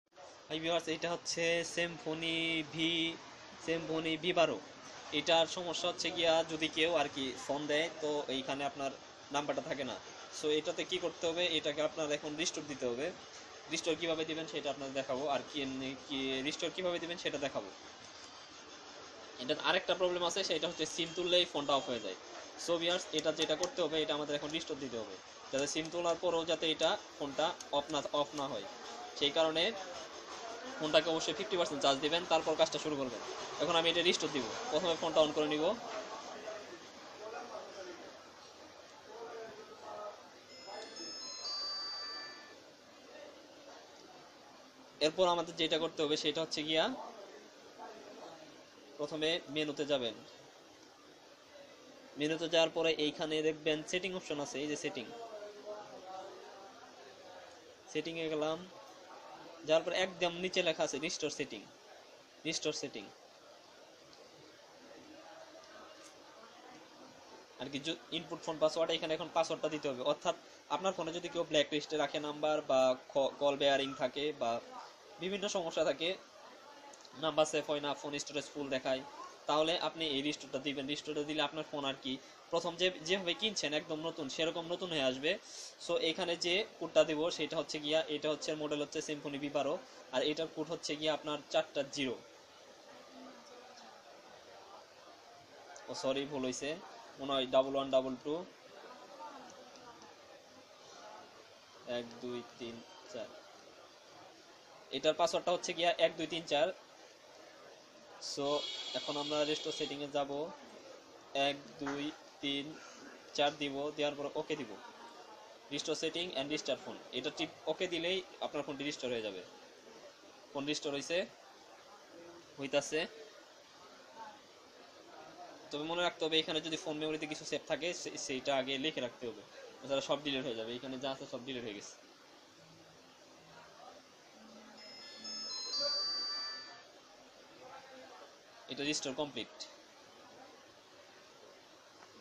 Symphony b12 reset phone
symphony b12 phone manual reset your phone need to a pass code so viewers see this video and reset your phone..